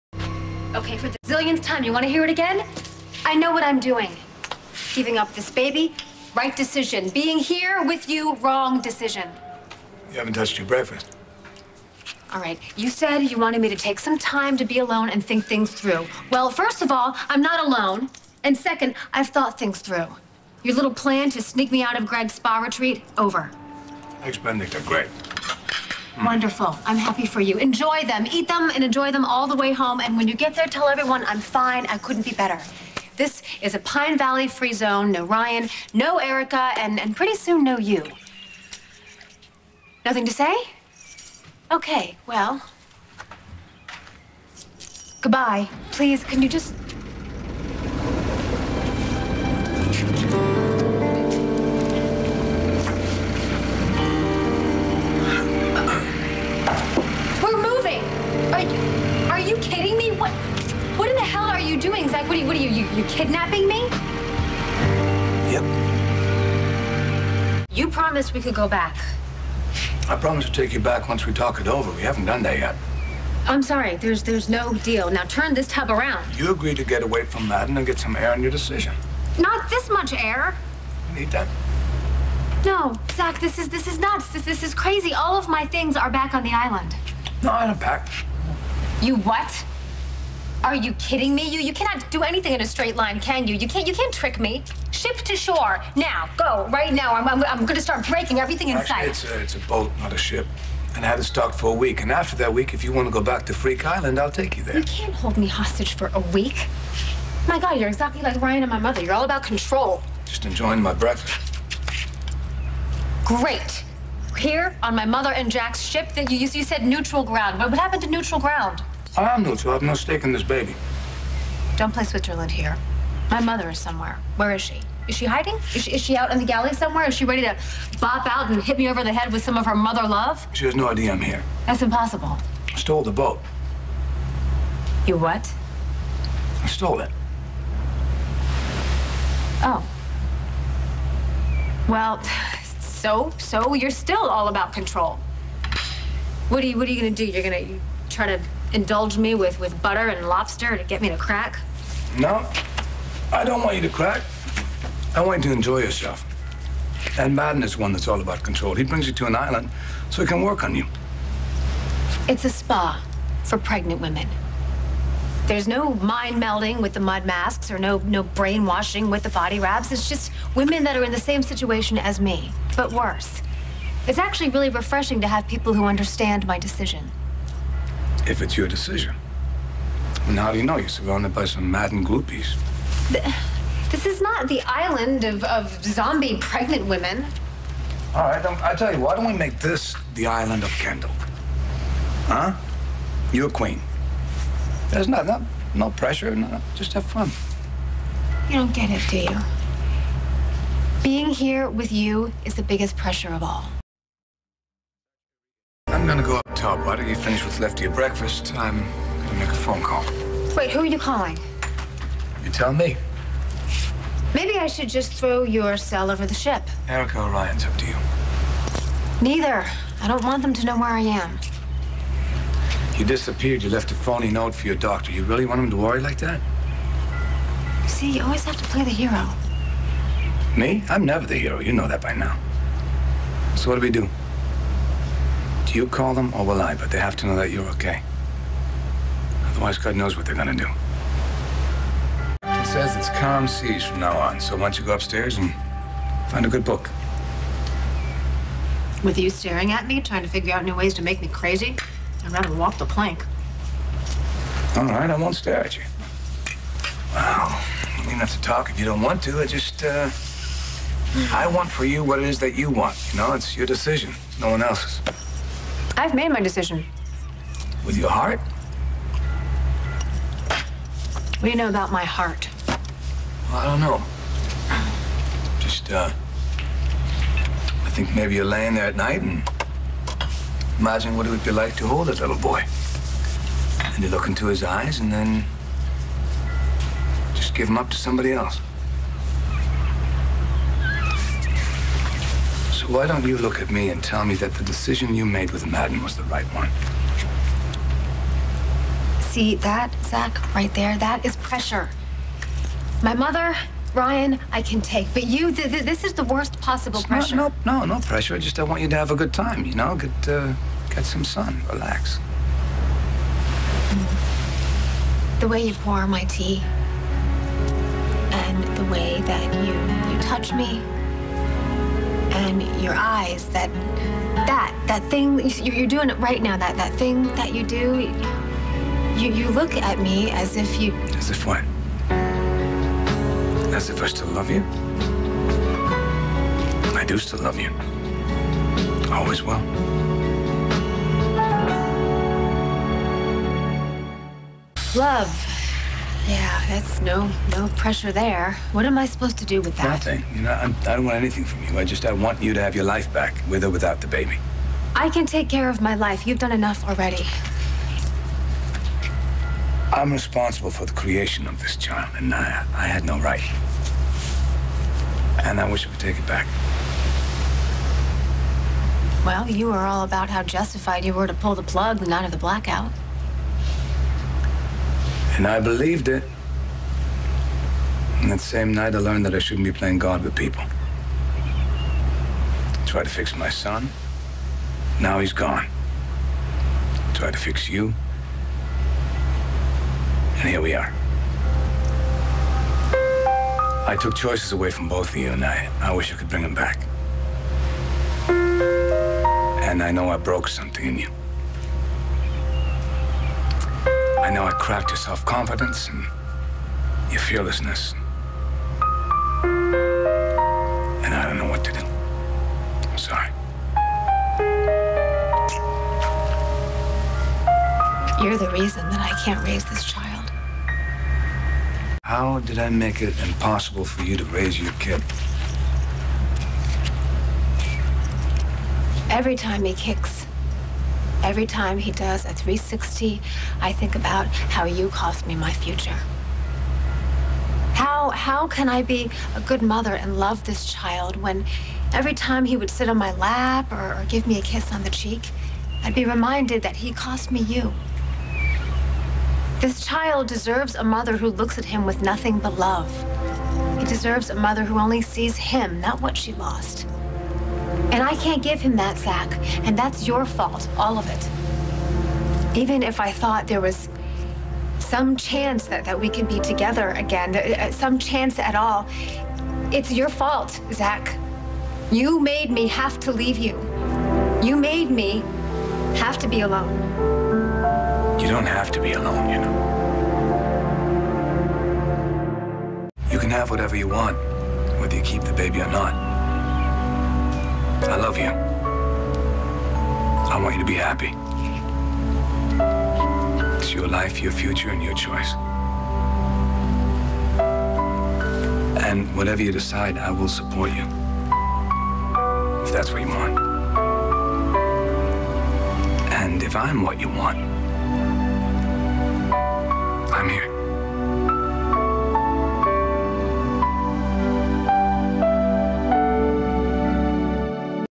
Zendall 4-28-06
Zach takes Kendall aboard Erica's yacht, which he stole to find her. Zach and Kendall talk about her decision to give up her baby. Zach tells Kendall that he still loves her.